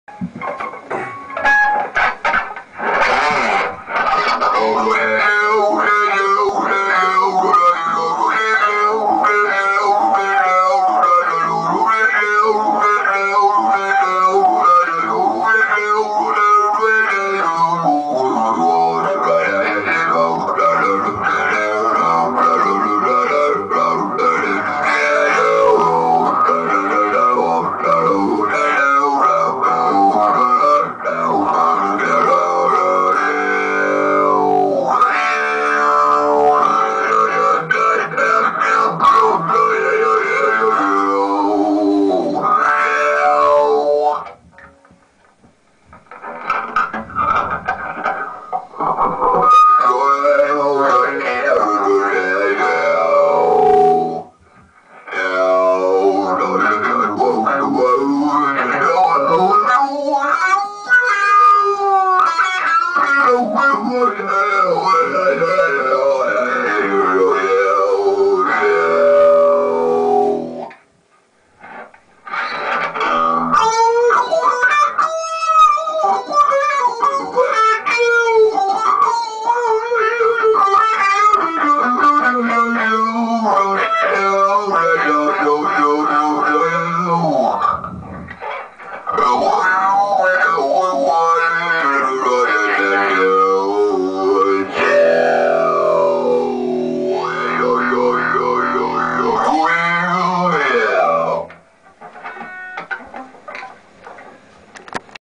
TalkBox Rocktron Test
Just a Quick test of the Rocktron Talkbox.
I used the LesPaul standard and JCM Slash Signature to support this test.